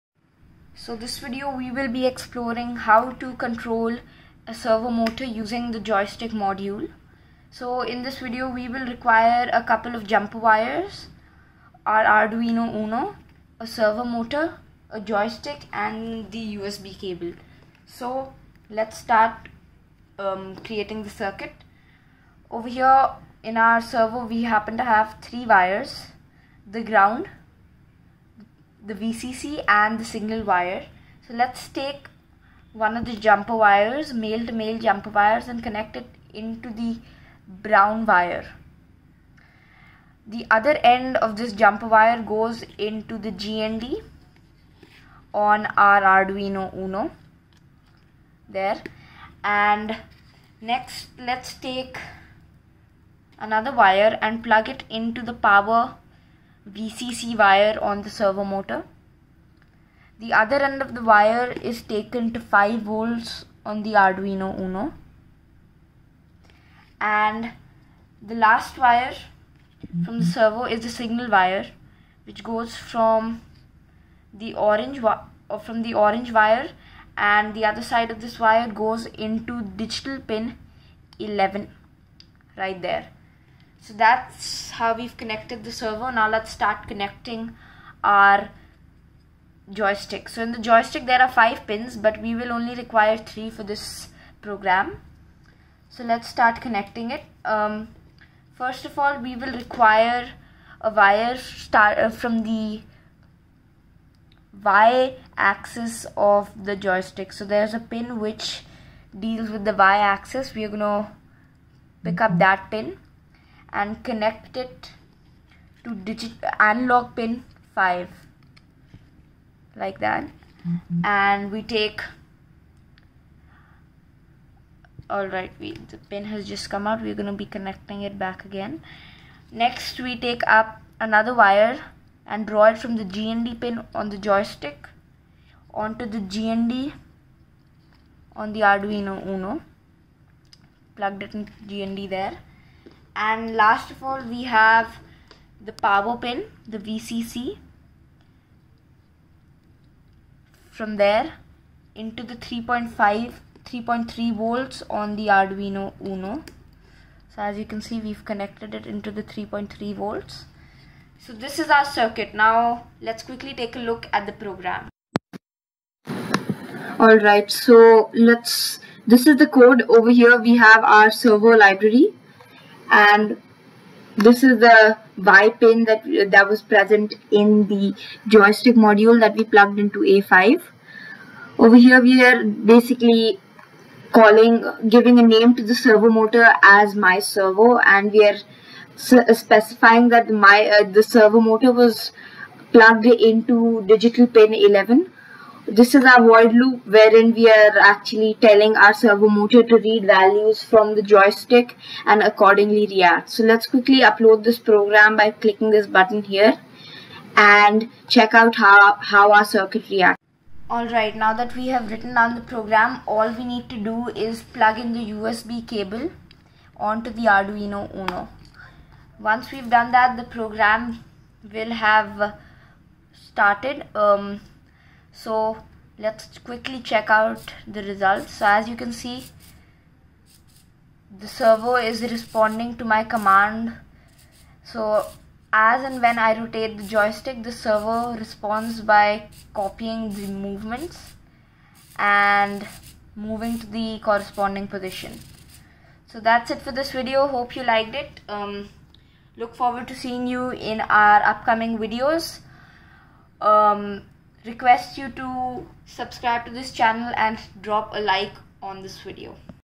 Controlling servo motor using a joystick | Arduino
Hi guys,
This tutorial were gonna checkout ways in which we can control the movement of the servo motor using a joystick module. It is extremely easy and explained thoroughly in the video. The code has also been explained properly. It is great for any level of expertise even if you are a beginner.